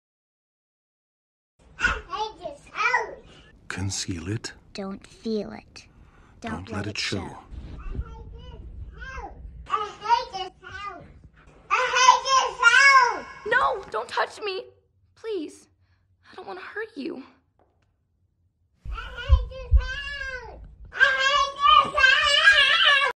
Anna Hates Her House (Animatic)
Imagine being quarantined for years and not knowing why.